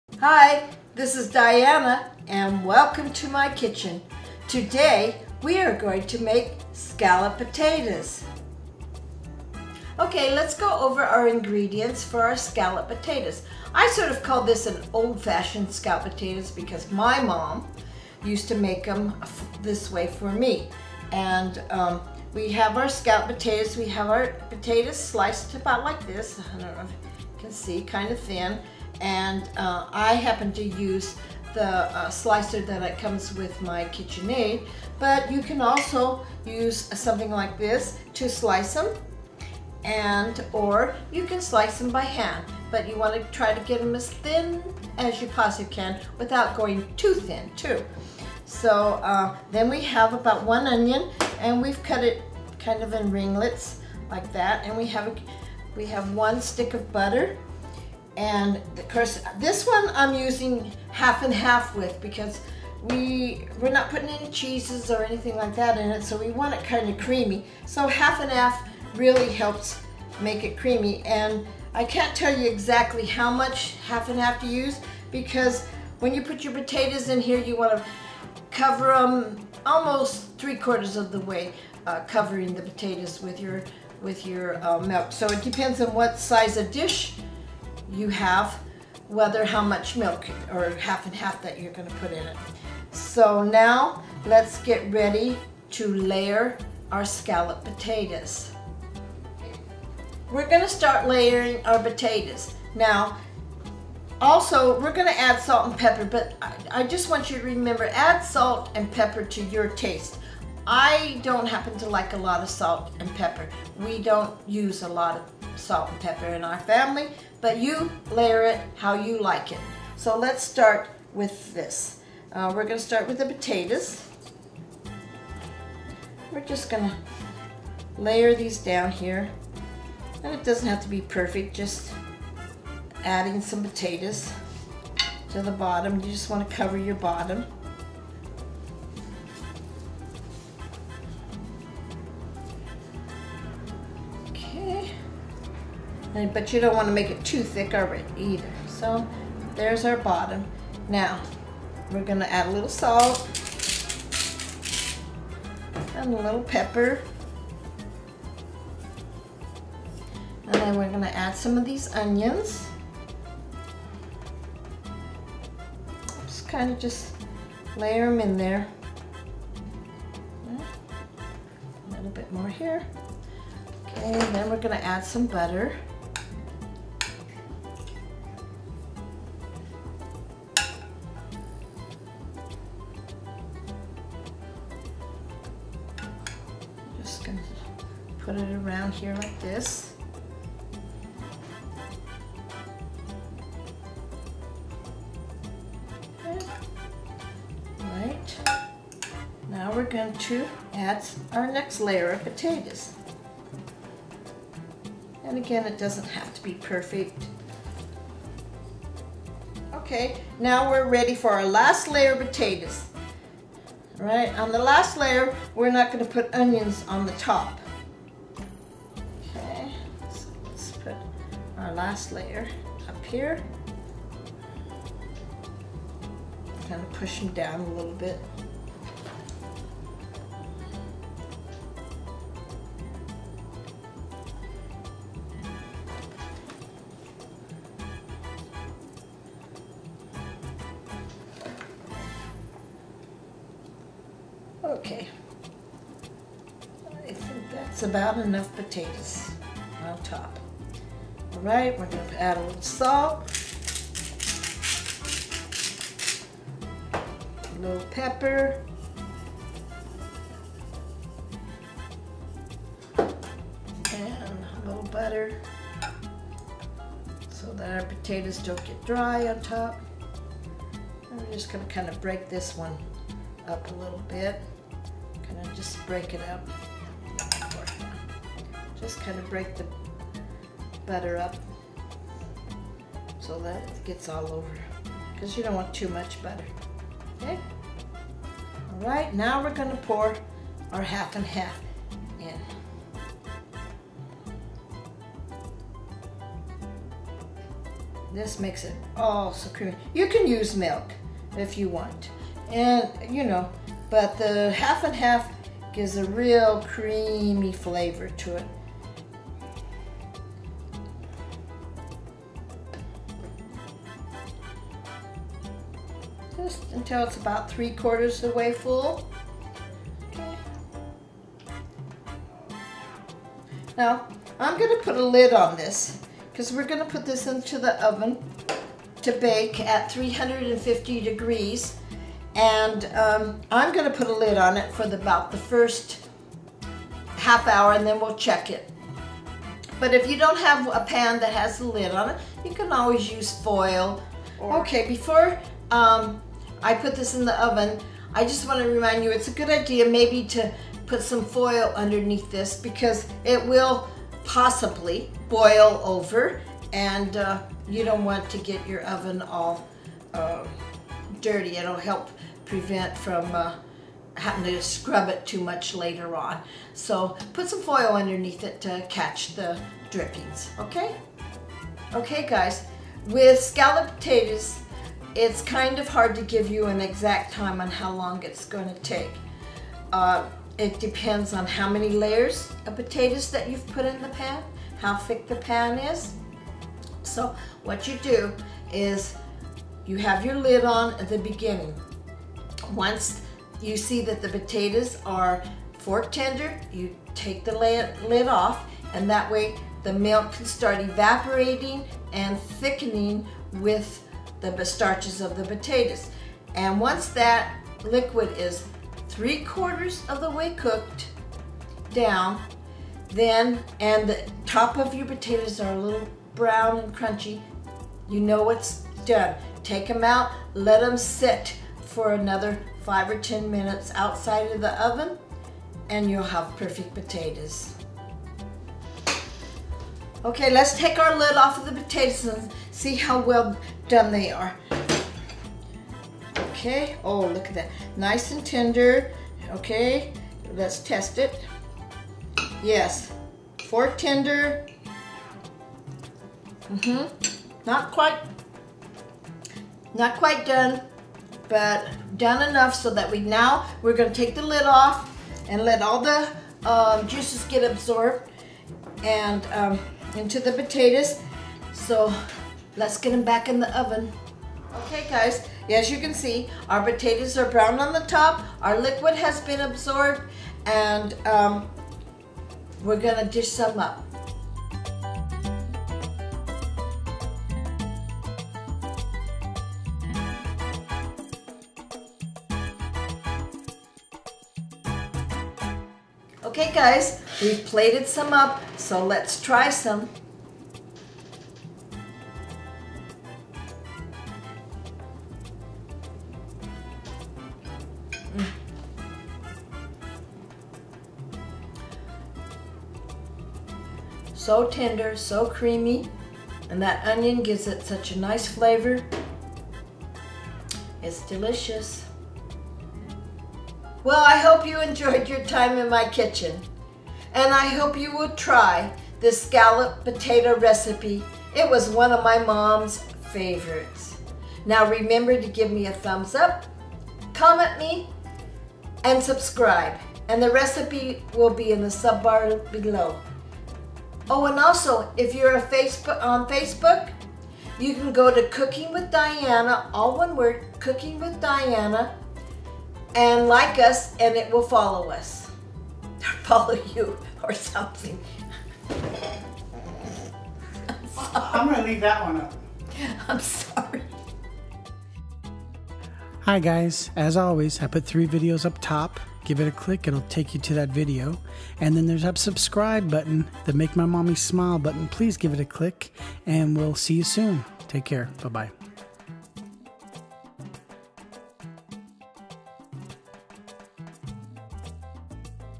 Old Fashion Scallop Potatoes episode #41
We will be posting the new address for the recipes soon.

Old Fashion Scallop Potatoes episode #41 of the CookingWithDiana Cooking Show.

On this episode of the "CookingWithDiana" Cooking show we will be showing how to make  Old Fashion Scallop Potatoes.  This has always been a Favorite recipe of Diana's maybe because she learned from her Mother.  But we wanted to share this easy Scallop Potato Recipe with you.  We hope you try and enjoy this recipe.  Let us know if you make it we would love to know how it turns out.